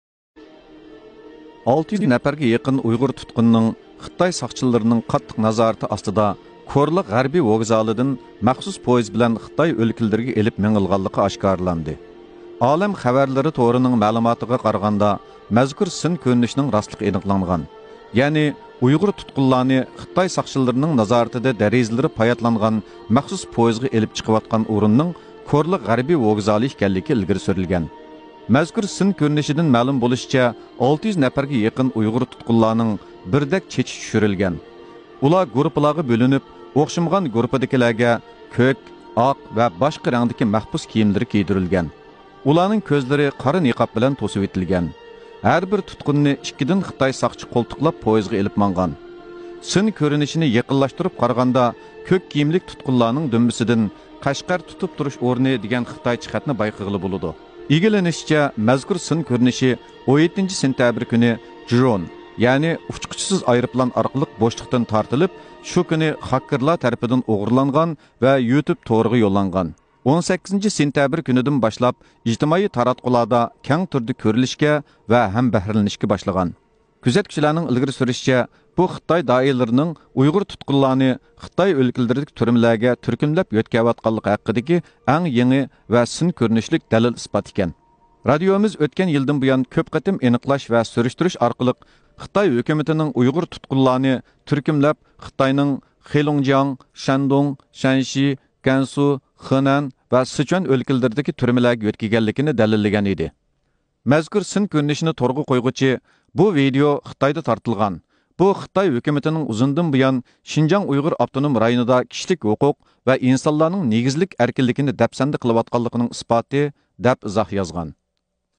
Çin suç üstü yakalandı
devlet terörü estiren Çin 600 fazla Uygur Türk’ü elleri kelepçiler ile başlarına çuval giydirerek bilinmeyen yere götürdü ifşa oldu